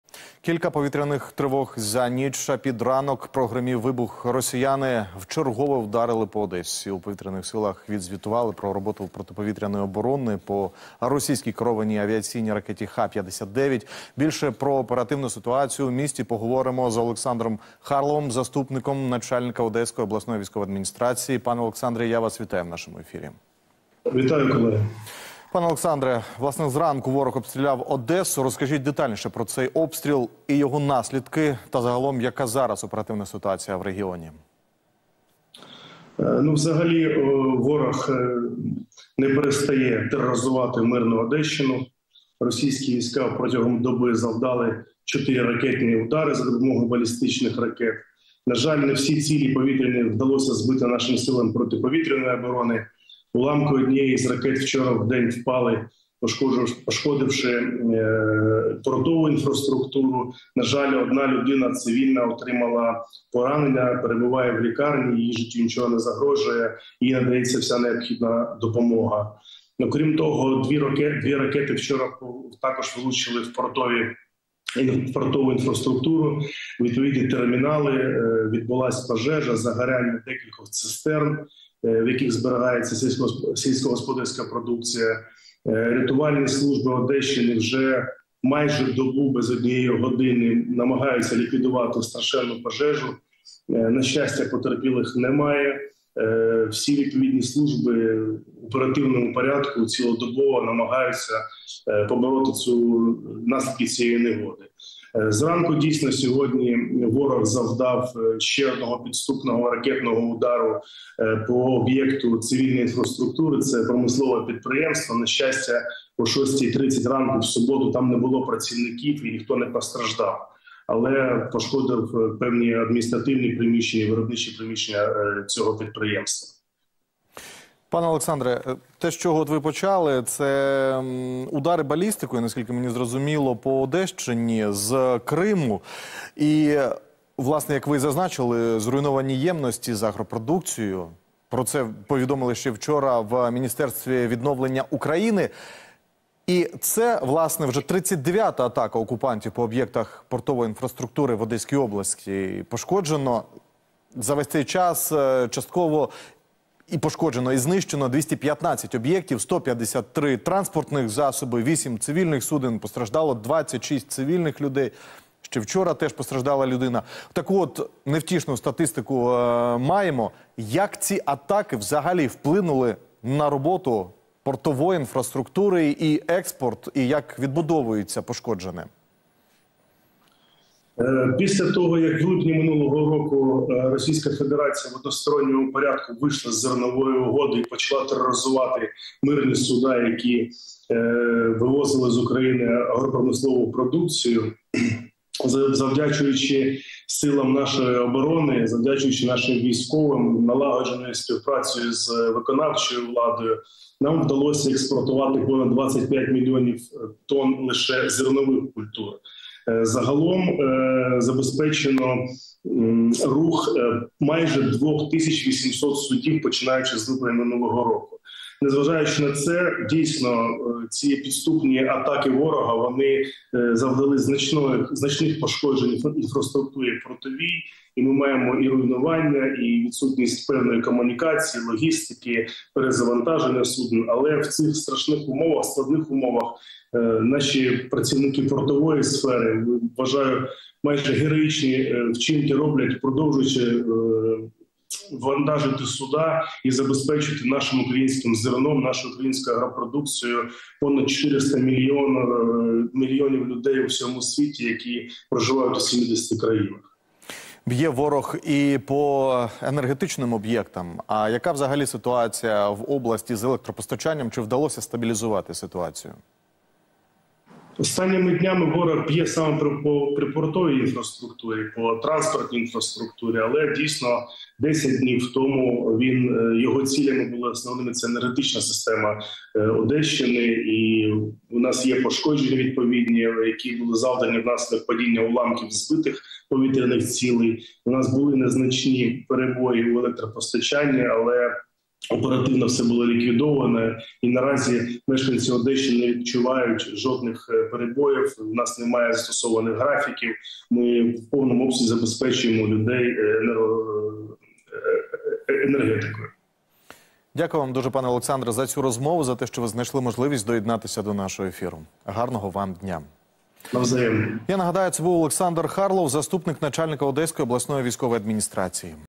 ПОЖЕЖА В ПОРТУ: в Одеській ОВА розповіли про наслідки ворожої атаки
На Одещині рятувальники вже добу намагаються загасити пожежу, яка спалахнула після ракетного удару по портовій інфраструктурі. Про це в ефірі телемарафону "Єдині новини" повідомив перший заступник голови Одеської ОВА Олександр Харлов.